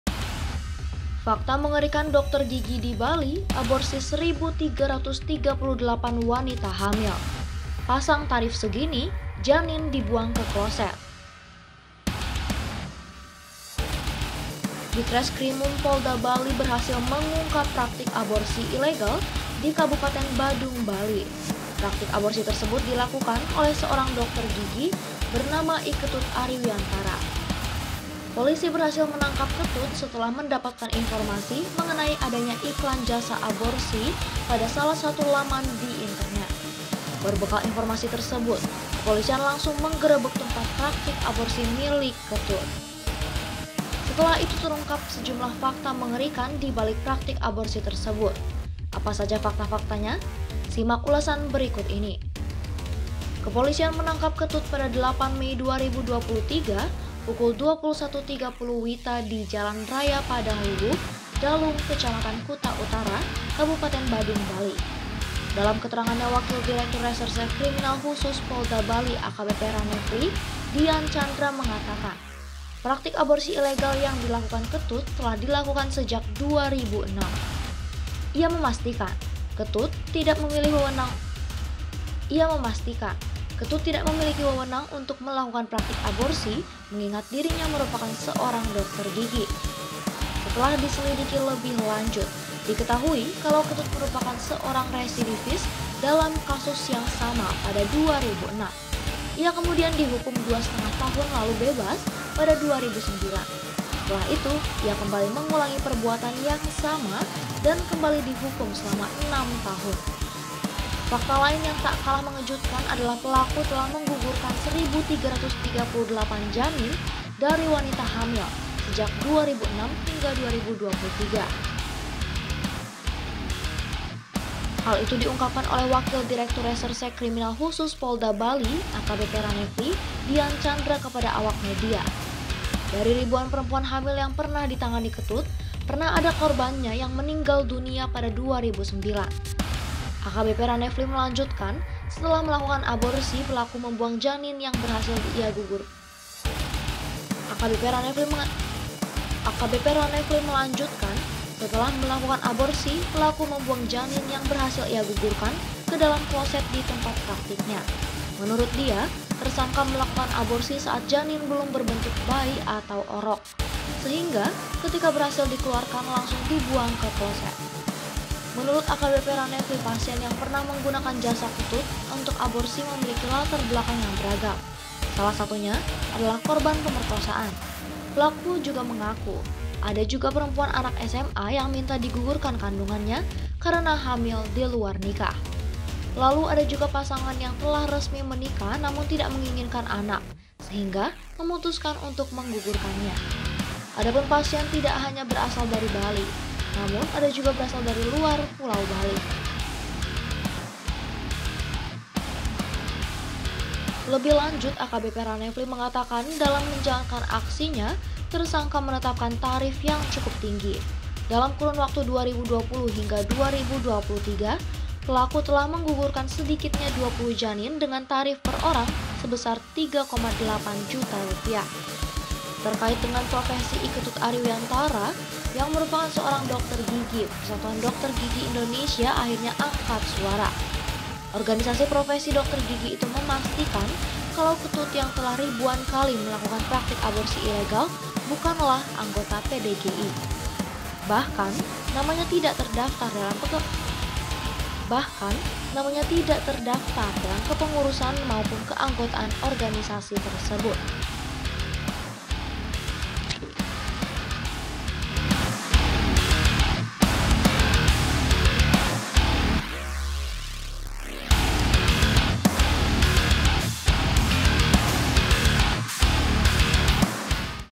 Fakta Mengerikan Dokter Gigi di Bali Aborsi 1.338 Wanita Hamil: Pasang Tarif Segini, Janin Dibuang k
Ditreskrimum Polda Bali berhasil mengungkap praktik aborsi ilegal di Kabupaten Badung, Bali. Praktik aborsi tersebut dilakukan oleh seorang dokter gigi bernama I Ketut Ari Wiantara.

Polisi berhasil menangkap Ketut setelah mendapatkan informasi mengenai adanya iklan jasa aborsi pada salah satu laman di internet. Berbekal informasi itu, kepolisian langsung menggerebek tempat praktik aborsi milik Ketut. Lihat selengkapnya di video.